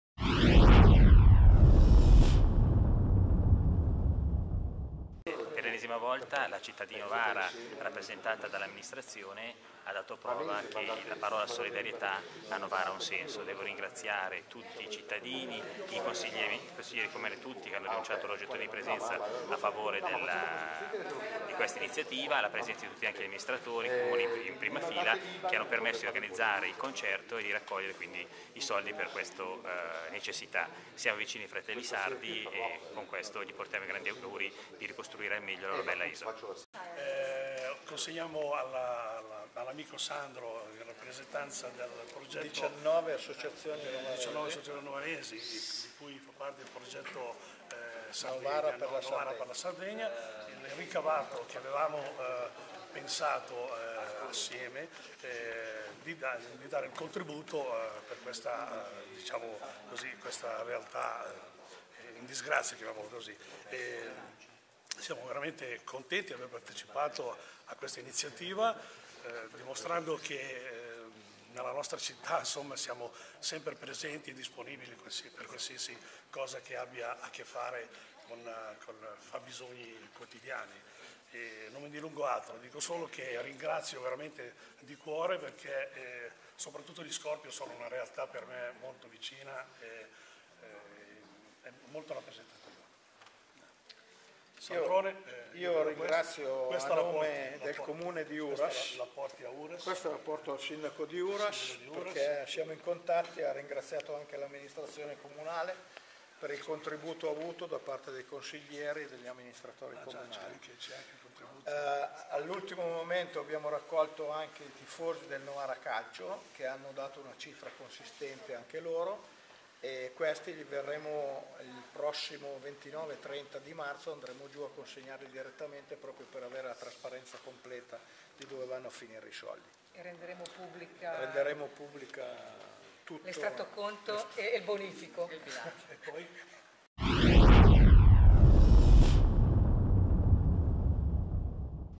"Novara per la Sardegna": parte del ricavato della Mezza Martona di San Gaudenzio all'iniziativa
Comunicato stampa Comune di Novara

Questa mattina nella sala del Consiglio Comunale a Palazzo Cabrino la UISP comitato provinciale di Novara ha consegnato gli assegni di benmeficienza legati all'edizione 2014 della Mezzamaratona di San Gaudenzio.
Sono stati premiati  gli istituti comprensivi scolastici della città che hanno aderito alla manifestazione con i gruppi più numerosi di podisti, offrendo anche gruppi di volontari che hanno collaborato all'organizzazione. Gli istituti premiati sono (in ordine decrescente di ammontare del contributo) l' I.C. "Achille Boroli", l'I.C. "Bottacchi", l'I.C. "Duca d'Aosta", l'I.C. "Bellini", l'I.C. "Fornara Ossola", l'I.C. "Baluardo Partgiiani" e l'I.C. "Levi Montalcini".
Nella stessa occasione l'organizzazione della Mezzamaratona ha devoluto 1250 euro al comitato composto da numerose associazioni di volontariato cittadine (Alice Novara Onlus, Associazione Novarese di Emergenza Sanitaria, Cad Novara, Casa Alessia Onlus, Cenacolo Dialettale, Cittadinovara, Claudia per l'Etiopia, Dj Running Team Novara, Non Solo Musica Novara, Nuares.it, Novara Soccorso, Il Quadrifoglio, I Ranat da Sant'Agabi, Rotellistica '93 Novara, Gruppo Scorpion Protezione Civile Novara, Telefono Azzurro Novara) che si è mobilitato per dare corpo ad un gesto di solidarietà nei confronti delle popolazioni alluvionate della Sardegna.

Alla consegna degli assegni, con il presidente del Comitato provinciale Uisp Nicolò Peretti Cucchi, erano presenti in rappresentanza dell'amministrazione l'assessore allo sport Giovanni Agnesina e l'assessore all'istruzione Margherita Patti, oltre al consigliere comunale Alessandro Negri, presidente della consulta dello sport e "patron" della Mezzamaratona